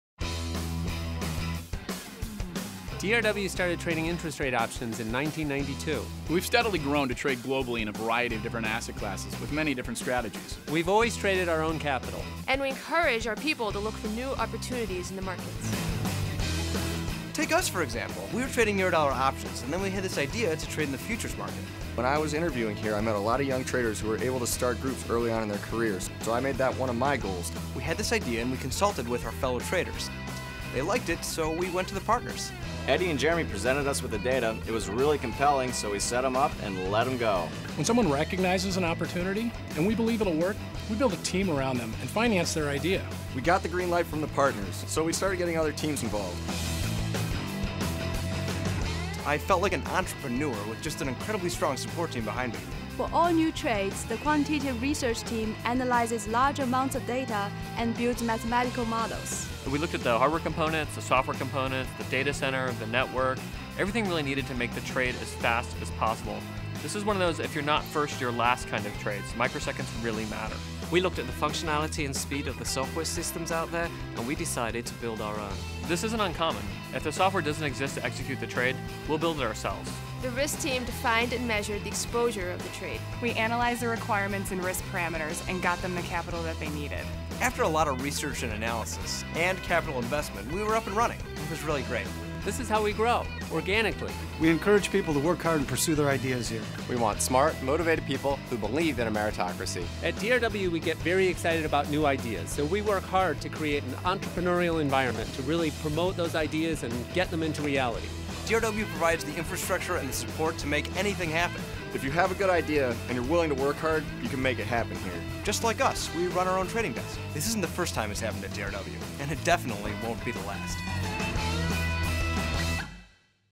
2011 - Recruiting Video
A quick look at opportunities, community and collaboration in DRW.